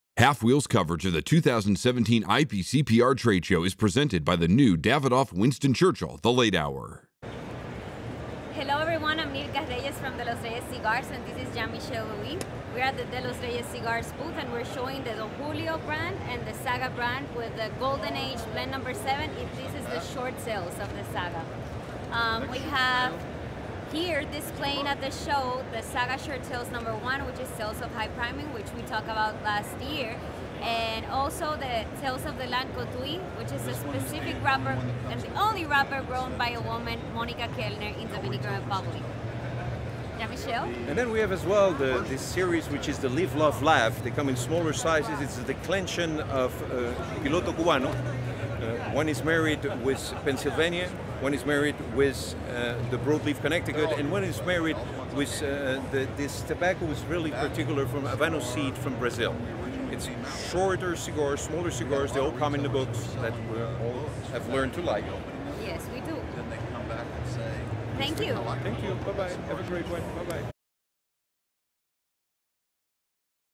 IPCPR 2017: De Los Reyes
halfwheel.com takes a look at the De Los Reyes booth at the 2017 IPCPR Convention & Trade Show in Las Vegas, Nev.

For more coverage of the IPCPR 2017 trade show, please

halfwheel's coverage of the 2017 IPCPR Convention & Trade Show is brought you by the Davidoff Winston Churchill The Late Hour.